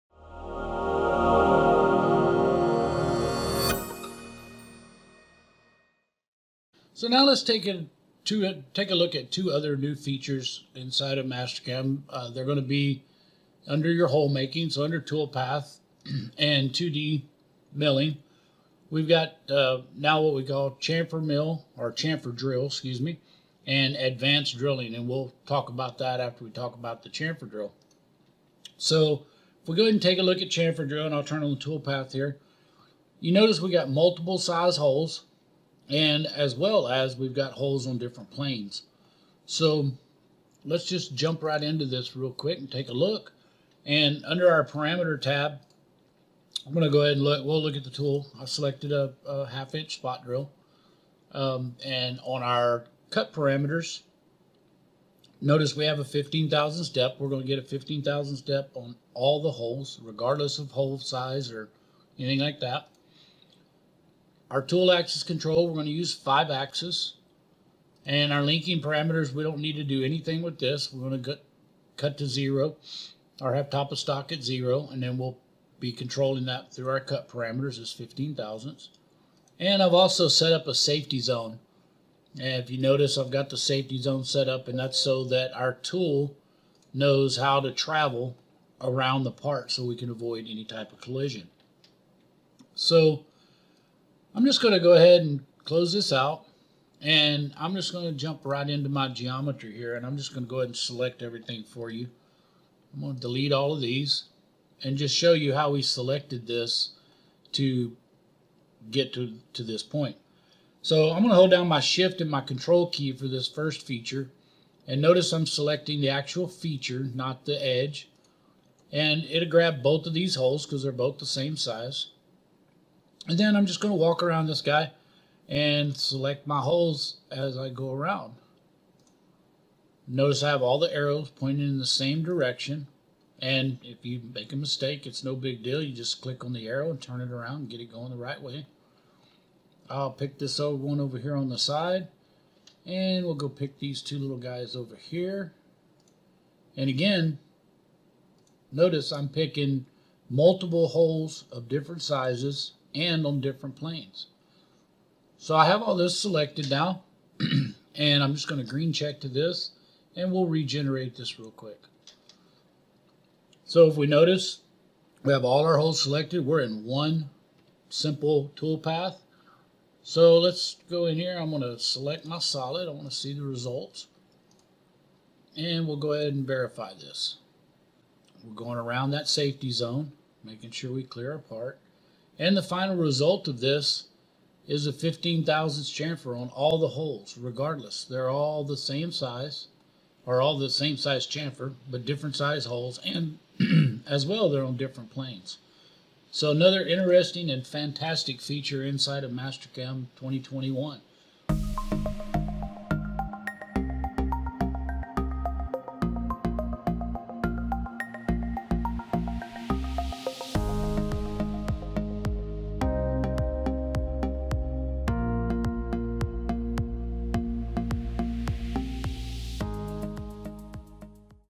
What's New Mastercam 2021 - Chamfer Drill Toolpath
Learn more about Mastercam products and support including post processors

Adding chamfers or spot drilling holes of various sizes on a single part just got easier with the new Chamfer Drill toolpath in @mastercam 2021.  Simply set the size of the desired chamfer and the selected face or edge sets the top of the feature and the size.  The toolpath sets the depth to achieve the desired chamfer result.  To make a simple center drill, simply use a chamfer size of zero or set a negative chamfer size and the center drill will set a suitable depth relative to the hole size.  Follow our expert Bo Rohde as he explains how to use this toolpath for holes on a part in various axes.